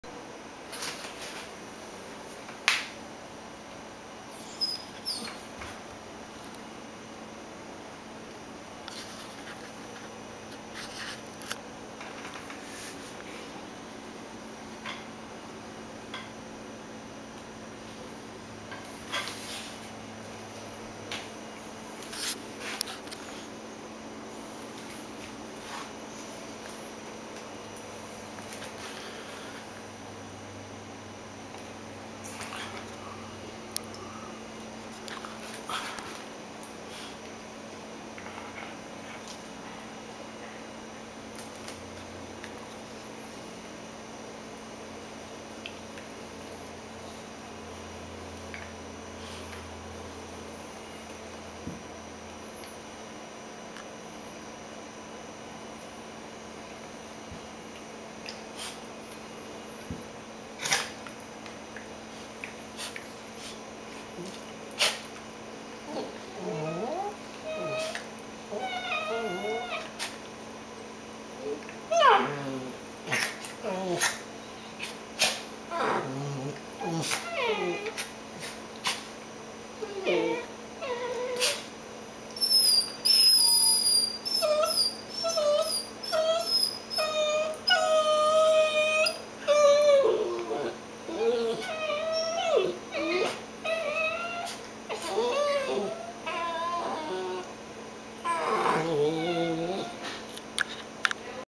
Peanut (dachshund) and Jig (cockapoo) chilling in bed before their morning nap
Peanut and Jig tired and playing with each other before their morning nap.